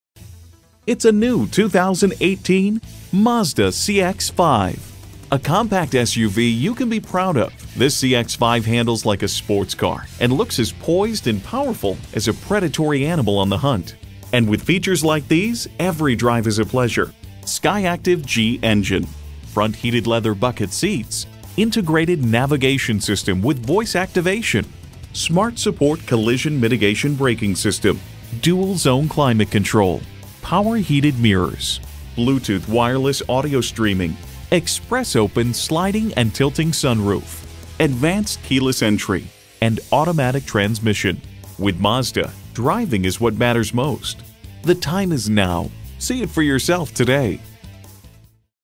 New 2018 Mazda CX-5 Baltimore, MD 5M816544 - SOLD
Year: 2018
Make: Mazda
Model: CX-5
Trim: Grand Touring
Engine: 2.5L 4-Cylinder
Transmission: 6-Speed Automatic
Color: White
Interior: Black
Stock #: 5M816544
VIN: JM3KFBDM2J0416544

Address:
6616 Baltimore National Pike